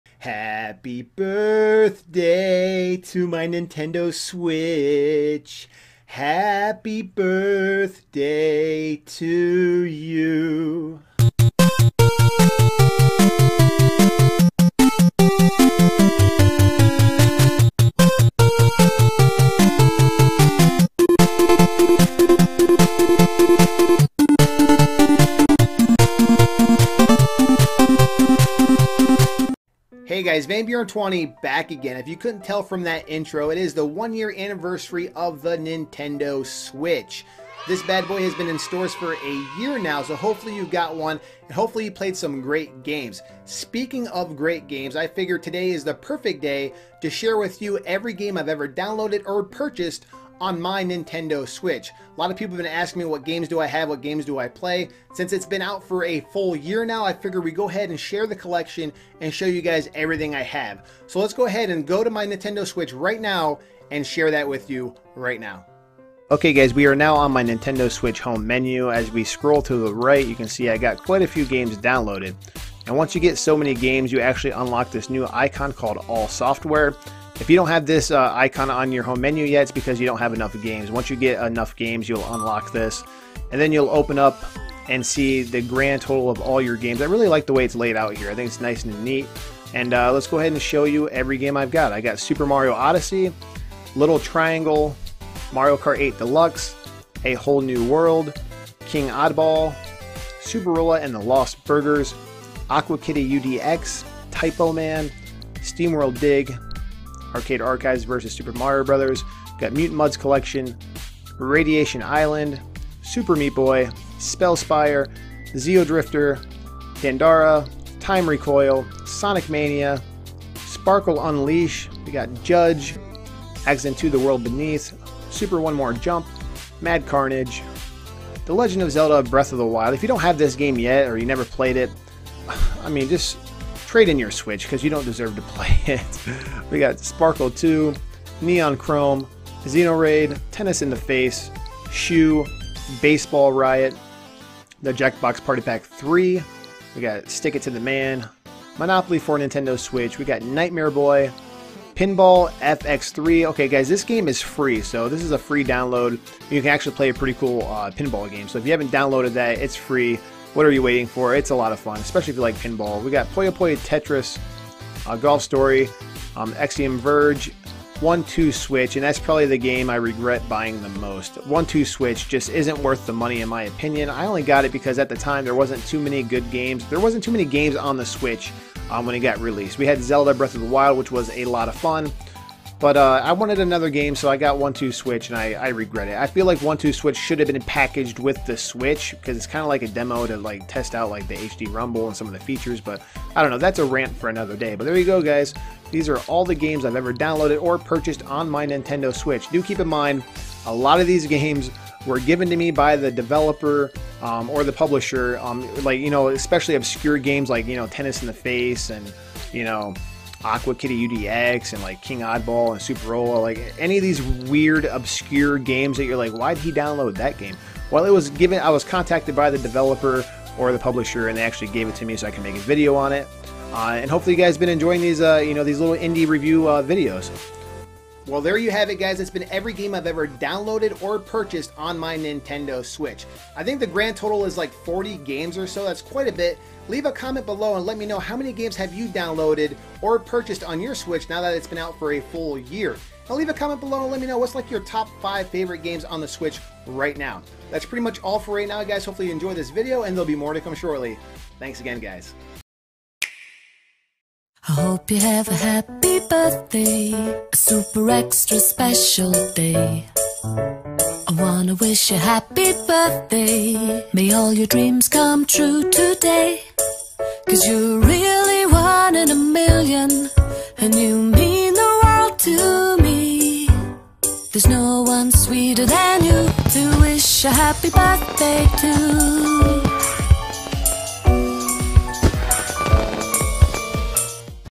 Happy Birthday Nintendo Switch! (Showing All My Games)
Today is the 1 year anniversary of the Nintendo Switch! Time to celebrate!

vanburen20
Ashtabula, Ohio 44005